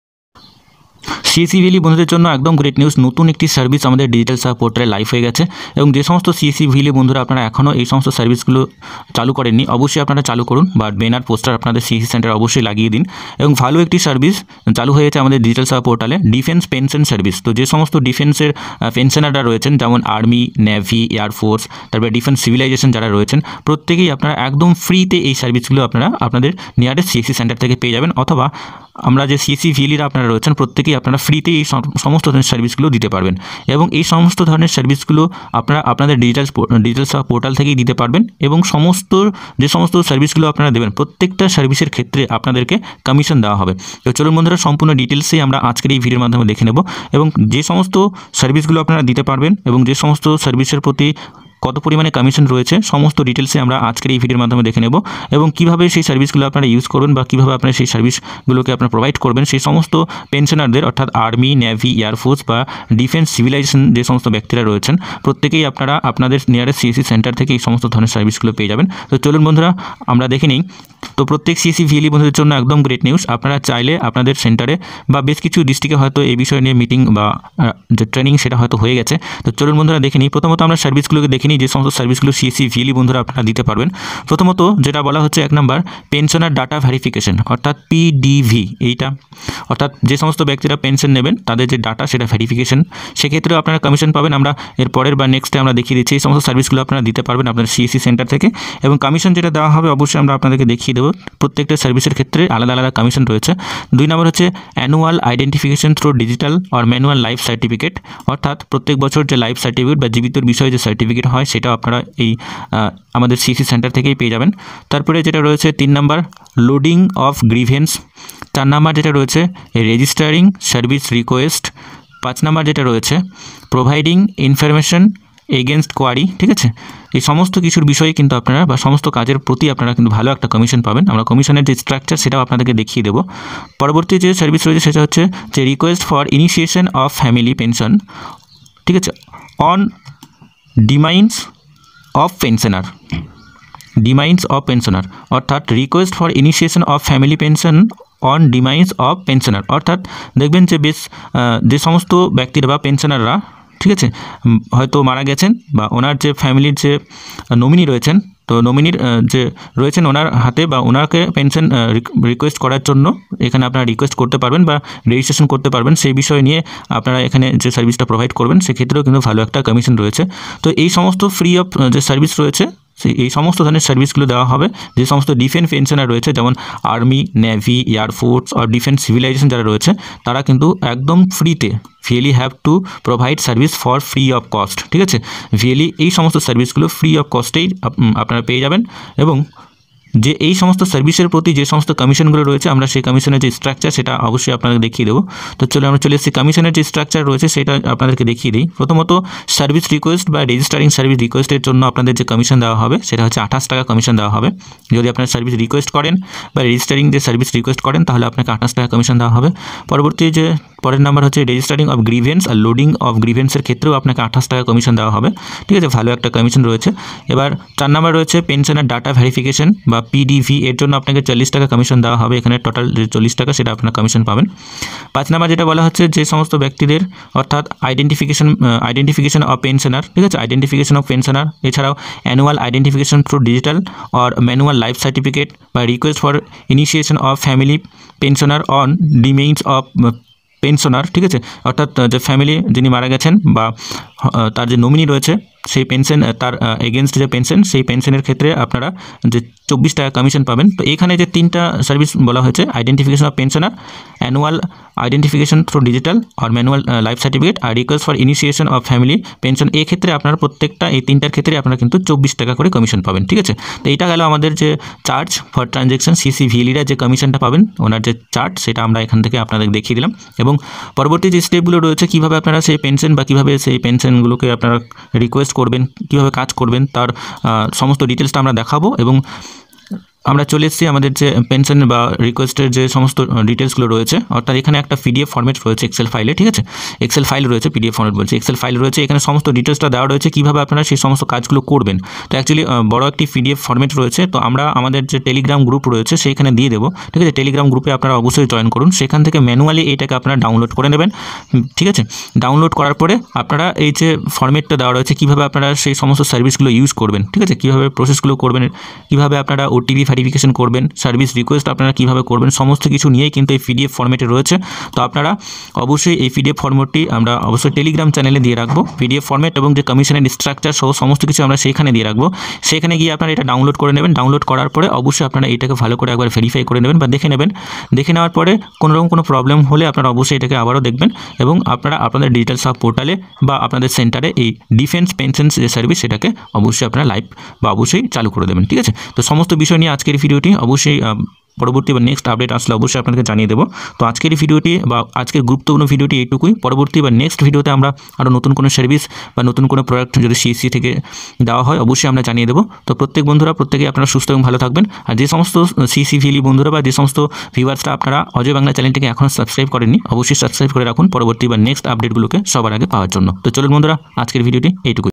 CSC New Service Update | Defence Pension Apply FREE of Cost | CSC VLE Commission Structure.
Good news CSC VLE Defence Pension Service available through CSC center.CSC New Service Update.Defence Pension Apply FREE of Cost.CSC VLE Commission Structure.